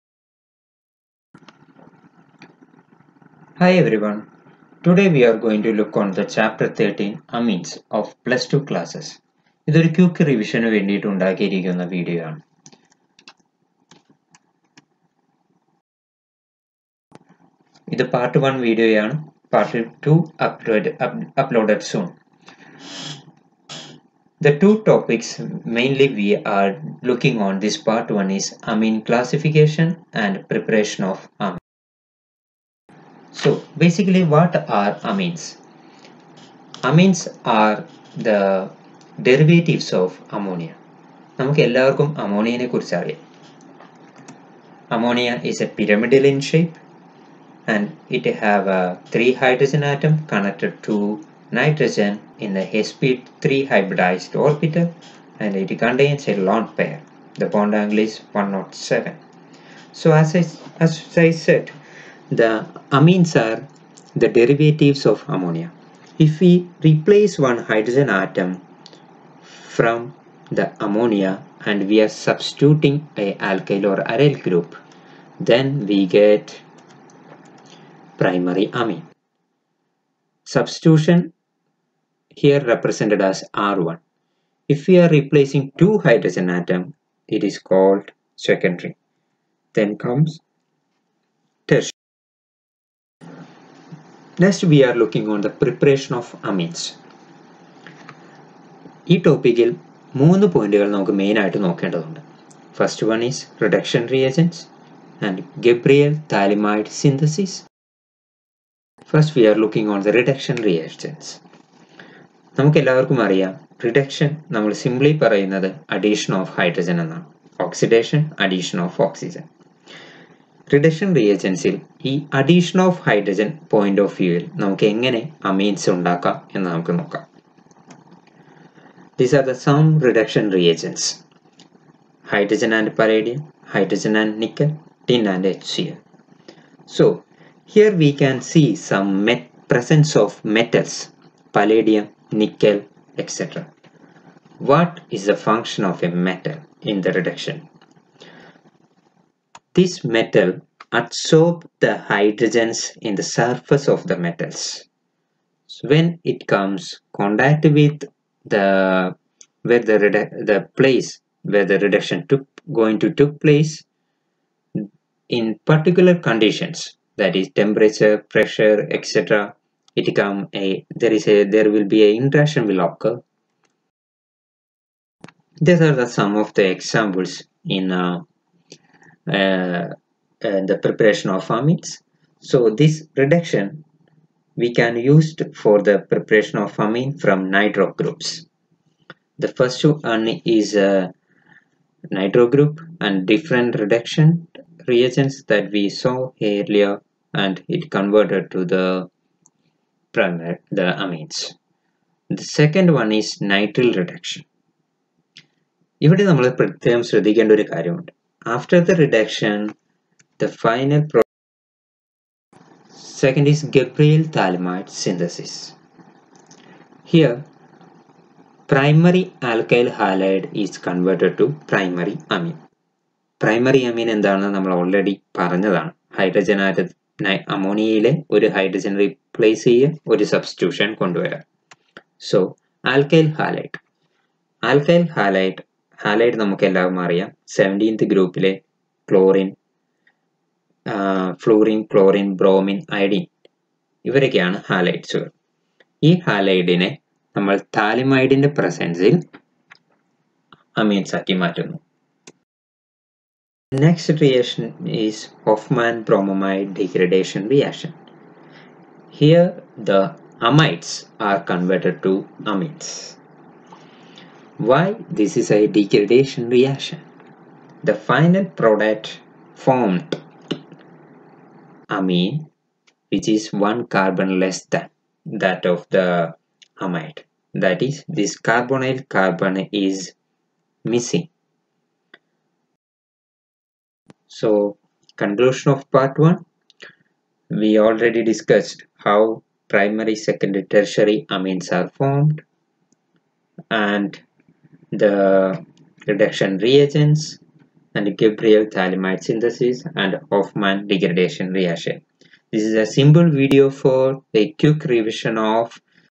Chapter 13 Amines in Malayalam | Quick Revision | PhiloEduce
Amines
What are amines?
How to Prepare Amines?
What are Primary, Secondary and Tertiary Amines?
What is Gabriel phthalimide synthesis ?
What is Hoffmann bromamide degradation reaction?